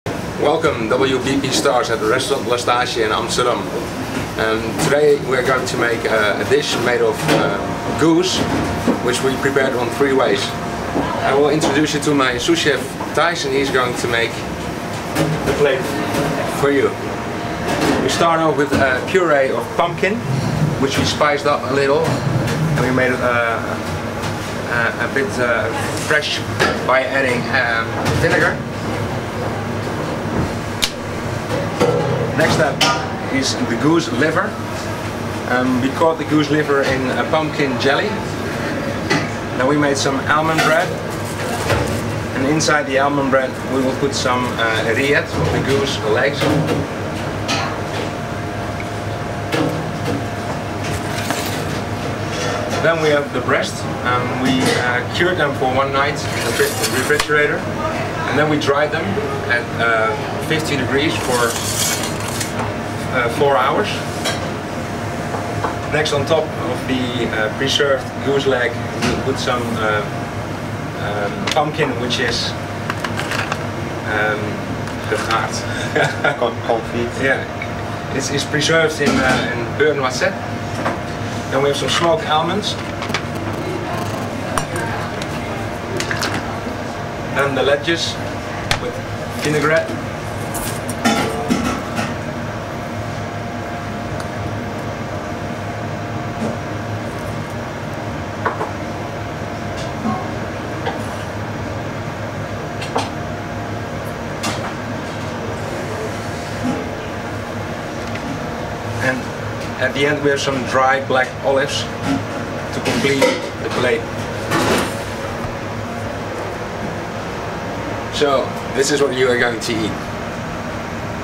Michelin star cooking at Lastage in Amsterdam
Chef Patron Rogier van Dam and Sous Chef Thijs Steur prepare a signature dish at the Michelin star Restaurant Lastage in Amsterdam (The Netherlands).